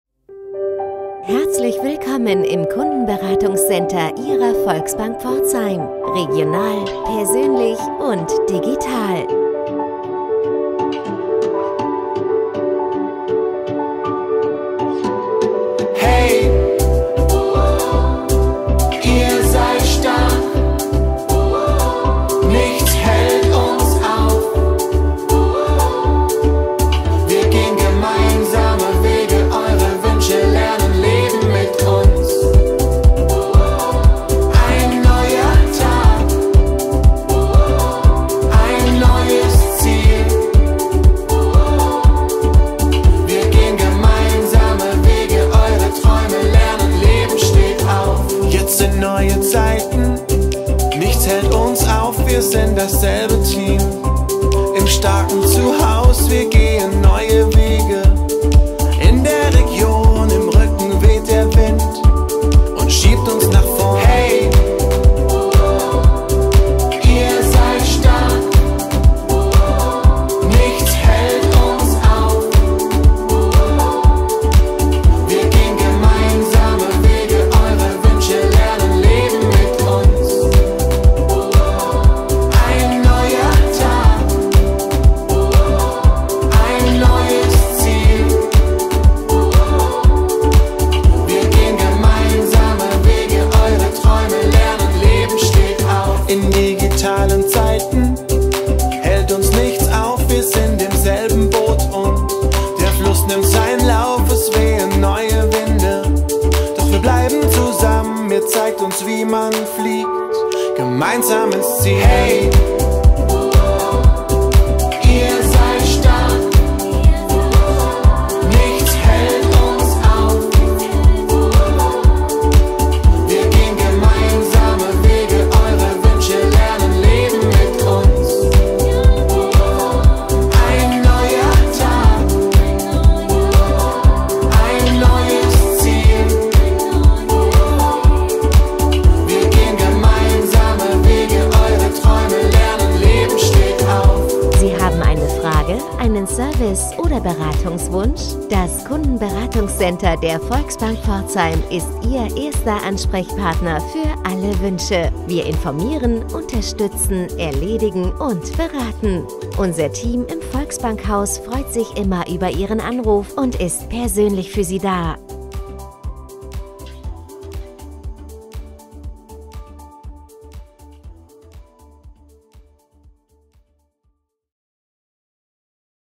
Das Kundenberatungs-Center der Volksbank Pforzheim
Wir sind für Sie da - regional, persönlich und digital!

In unserem Kundenberatungs-Center erreichen Sie kompetente und Berater Ihrer Volksbank Pforzheim.
EINFACH - ein Anruf genügt und wir kümmern uns um eine Lösung
BEQUEM - erledigen Sie Ihre Bankgeschäfte wo immer Sie auch sind
SCHNELL - Sie erreichen uns persönlich an fünf Tagen in der Woche
FLEXIBEL - Sie können Ihre Wünsche per E-Mail oder auf dem Anrufbeantworter los werden
PERSÖNLICH - bei uns betreuen Sie qualifizierte Berater
SICHER - mit Ihren Zugangsdaten können die Mitarbeiter Sie eindeutig identifizieren und Ihre Aufträge entgegennehmen

Sie haben eine Frage, einen Service- oder Beratungswunsch?

Rufen Sie uns einfach an - wir informieren, unterstützen, erledigen und beraten.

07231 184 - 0

Mit unserem Kundenberatungs-Center im VolksbankHaus sind wir von Montag bis Freitag von 08:00 Uhr bis 18:00 Uhr persönlich für Sie da! Außerhalb der Öffnungszeiten erreichen Sie uns digital. Natürlich vereinbaren wir auch gern Beratungstermine in unseren Filialen für Sie. Ihre Zufriedenheit ist unser Anspruch - deshalb freuen wir uns immer über ein Feedback von Ihnen und fragen bei jedem Anruf nach Ihrem Erlebnis.

Voraussetzungen:
- ein für das Kundenberatungs-Center Ihrer Volksbank Pforzheim freigeschaltetes Konto
- spezielle Zugangsdaten für das Kundenberatungs-Center, damit die Mitarbeiter Ihrer Volksbank Pforzheim Ihre Identität auch am Telefon überprüfen können
- Telefon (egal ob modernes Smartphone oder ältere Telefon)

Wir freuen uns auf Ihren Anruf!